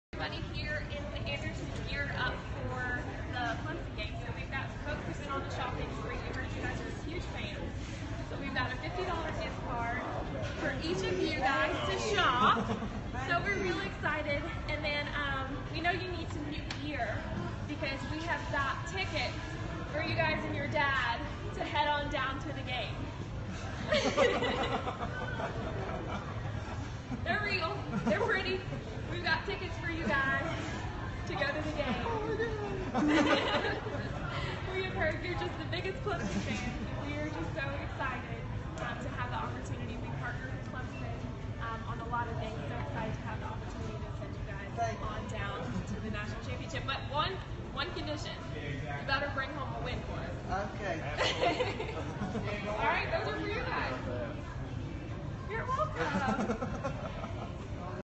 TigerNet.com - Clemson fan with cerebral palsy surprised with tickets to national championship
Sixteen-year-old Logan Wires will be going to Tampa courtesy of Academy sports.